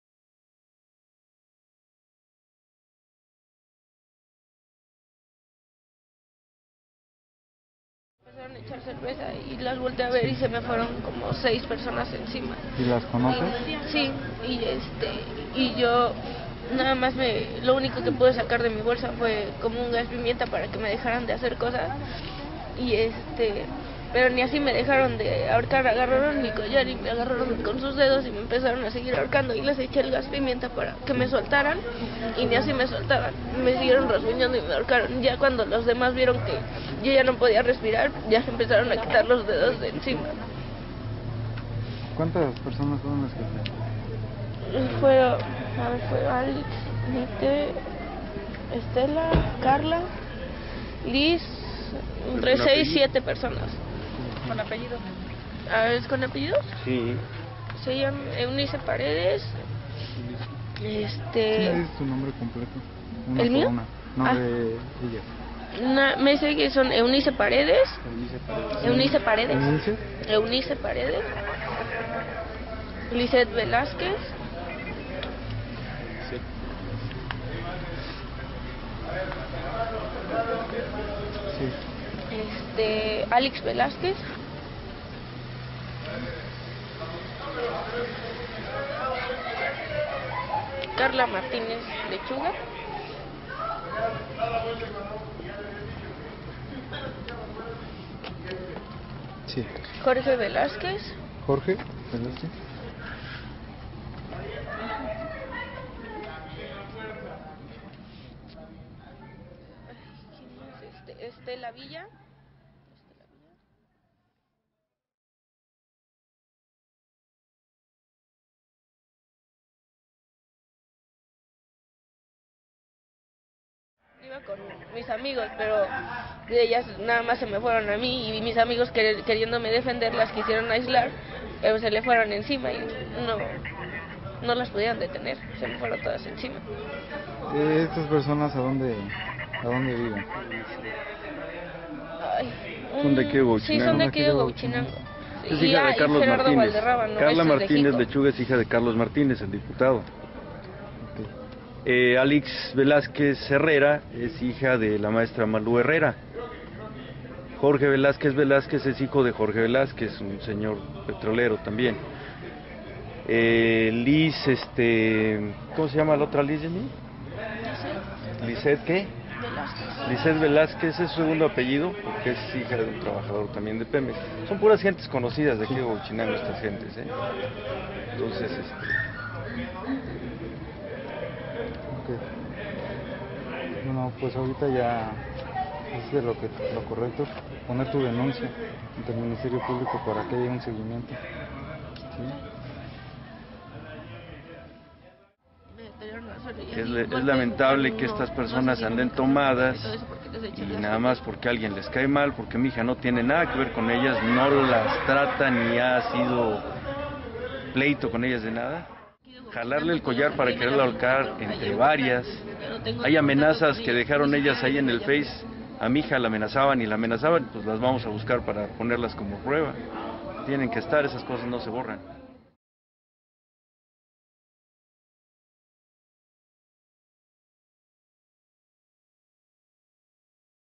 Hija de diputado local del PRD ahorca a vecina de Huauchinango
En un video subido a Youtube una joven detalla que la hija de Carlos Martínez Amador, ex presidente municipal de Huauchinango y próximo secretario General del partido del Sol Azteca, le arrojó cerveza, la araño y con su propio collar la ahorcó